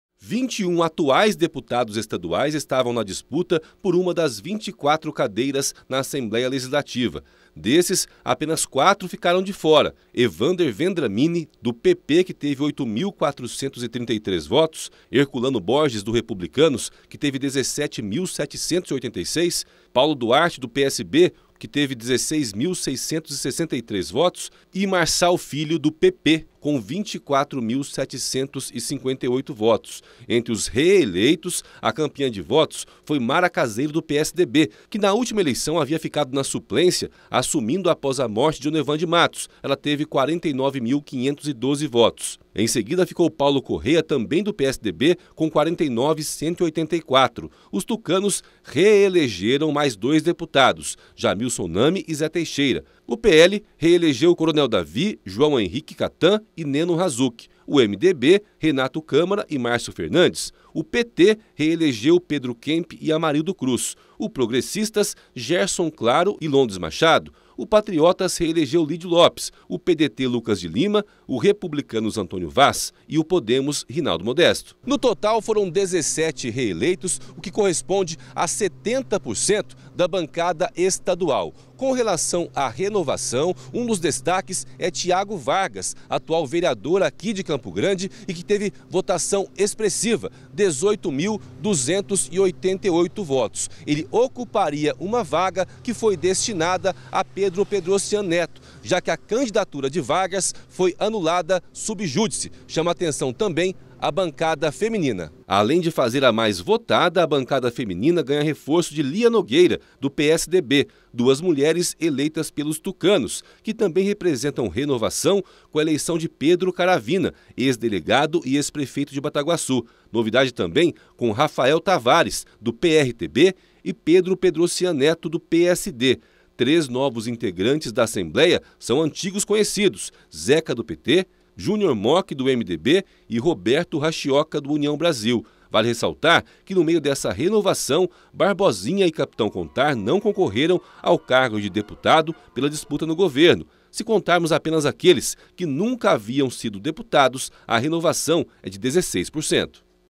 Em MS, urnas reprovaram apenas quatro dos 21 deputados estaduais que tentaram a reeleição
Ao contrário da eleição passada, quando nenhuma mulher foi eleita, desta vez a Assembleia Legislativa terá duas representantes. Mas a renovação na casa de leis foi pequena.